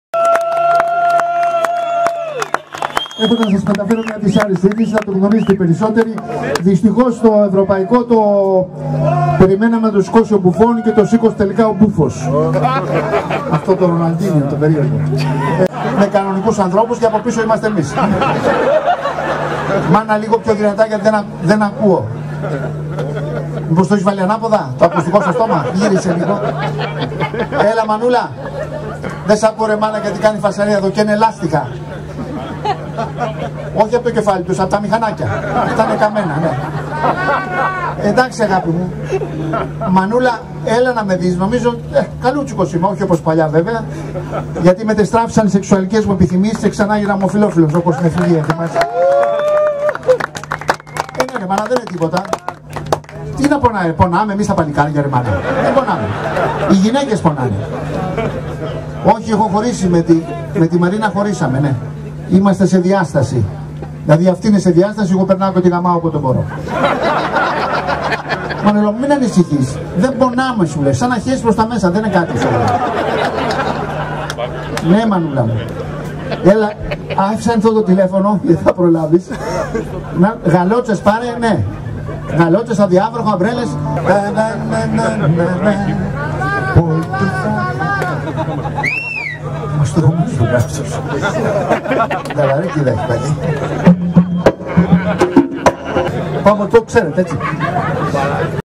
Τζίμης Πανούσης τηλέφωνο στην μάνα του !
Τζίμης Πανούσης για την κατάκτηση του Champions League από την Ρεάλ Μαδρίτης, μιλάει με την μάνα του στο τηλέφωνο και στιγμές από την συναυλία στην 36 Πανελλήνια Συγκέντρωση Μοτοσυκλετιστών στα λουτρά Νέας Απολλωνίας «Περιμέναμε να το σηκώσει ο Μπουφόν και το σήκωσε ο μπούφος!»